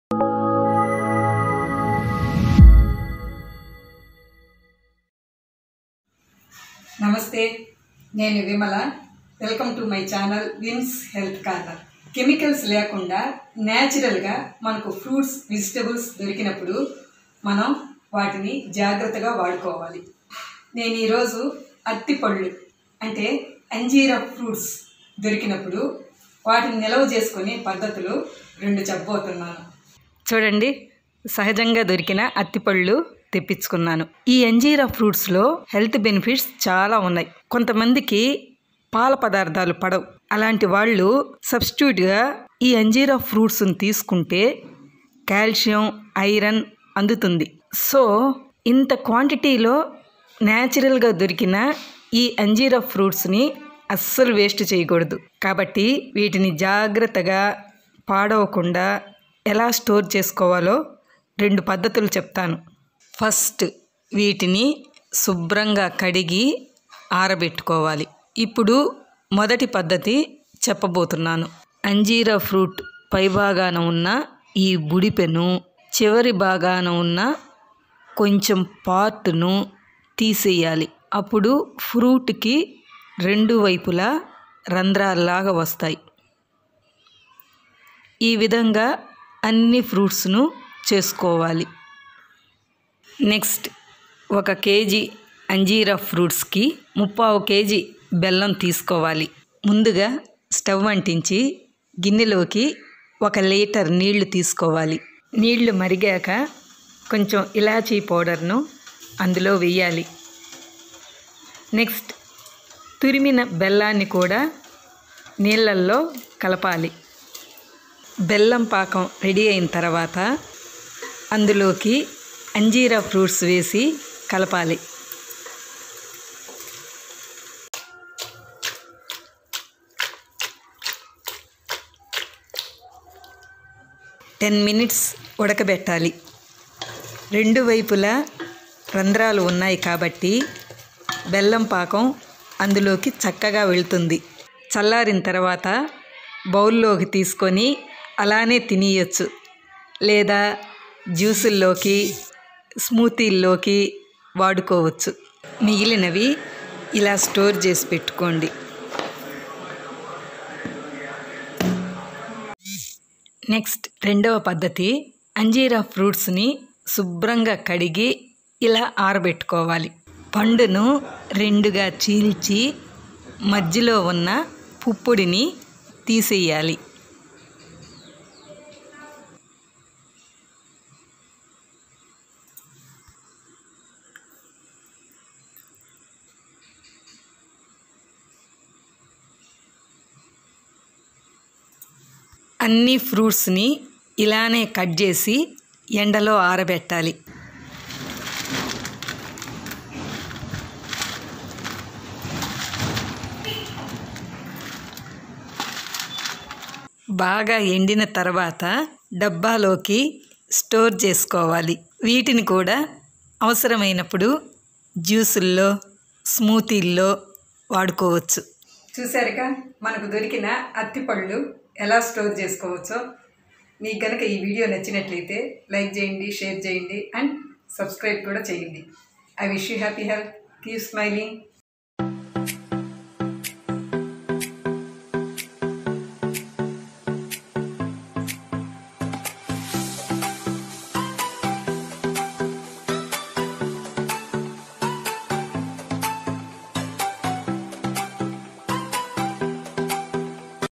How To Store Fig Fruits At Home | అత్తి పండ్లు | అంజీర పండ్లు | Athi | Anjeera | Fig Benefits
Hello Health Lovers,

Fruits are always helpful to maintain a healthy life, especially the Figs have many more benefits to maintain our health track.

Learn two different methods to store the fig fruits for more days. In this video first, we will discuss how to store figs with jaggery, and in the second method, we will prepare dryfigs.

Follow the easy steps shown in the video and gain your health bonus easily.